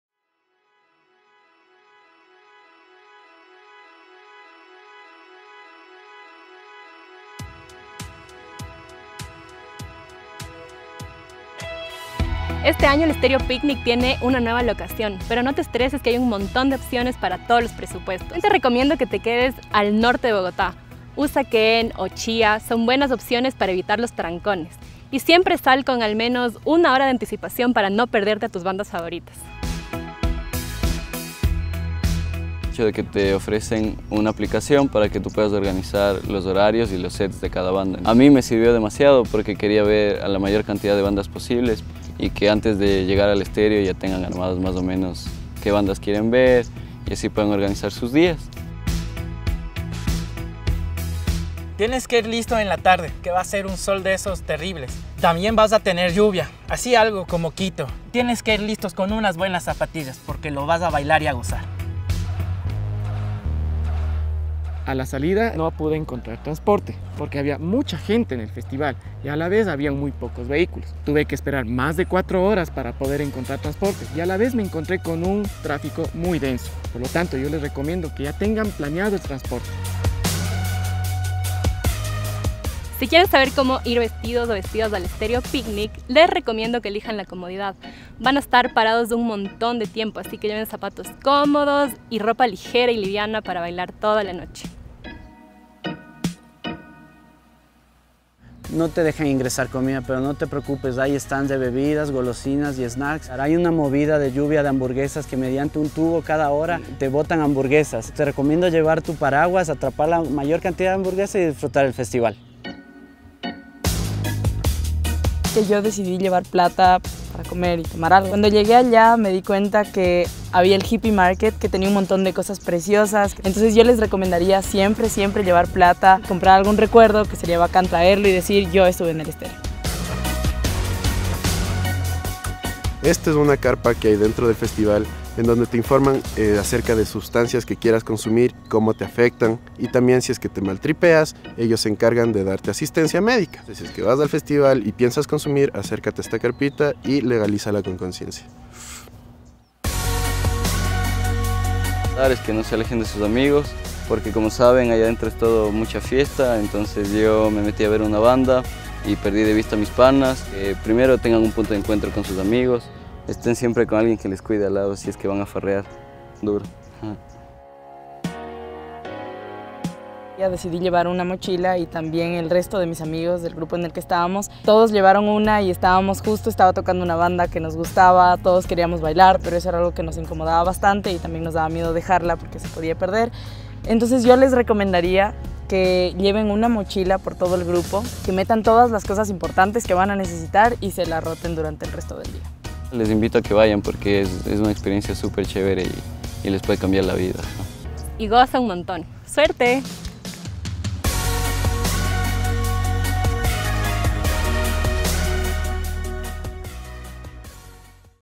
Guía de Supervivencia al Picnic (RC para el FEPX)
Se acerca la décima edición del Festival Estéreo Picnic, por eso les traemos 10 tips festivaleros para los nuevos creyentes del FEPX.

¡Gracias a todos los creyentes que nos contaron sus experiencias en ediciones anteriores del Picnic!

Dirección: Andrés F. Flores

Cámara: Ricardo Sempértegui

Sonido: Sebastian Cabascango y David Torres

Arte: Rachel Proaño y Melanie Guerra

Asistente de Producción: Alejandro Adriano

Creyentes:
Carla Vera
Alejandro Molina
Armando Chavez
Angel Chavez
Emiliano Proaño
Juliana Jiménez
José Luis Pacheco

RC 2019